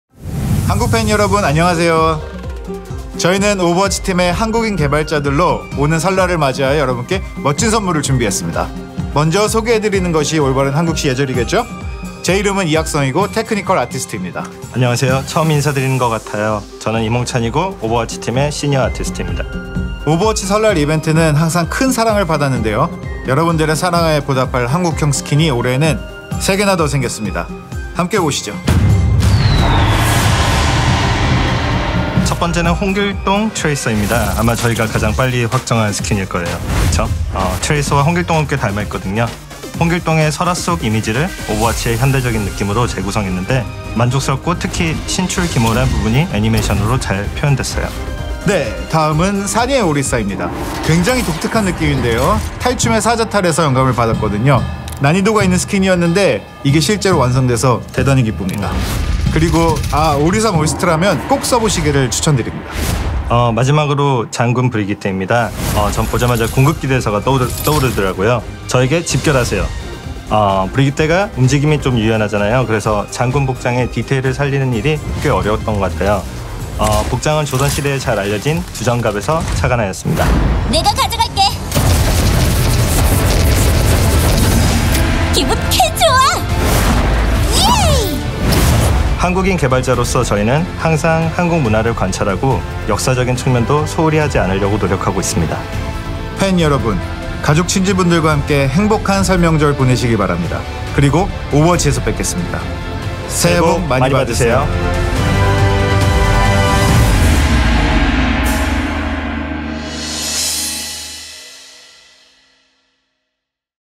개발자 업데이트 | 오버워치 황금 돼지의 해 이벤트
오버워치에서 황금 돼지의 해를 맞이하세요!

오버워치 개발팀의 한국인 개발자 테크니컬 아티스트 이학성님과 선임 아티스트 임홍찬님이 오버워치 설날 이벤트와 새 한국 스킨에 대해 이야기합니다.

00:20 – 오버워치 설날 이벤트에 대해
00:34 – 이번 이벤트에서 선보이는 한국 스킨들
01:27 – 오버워치 개발팀의 새해 인사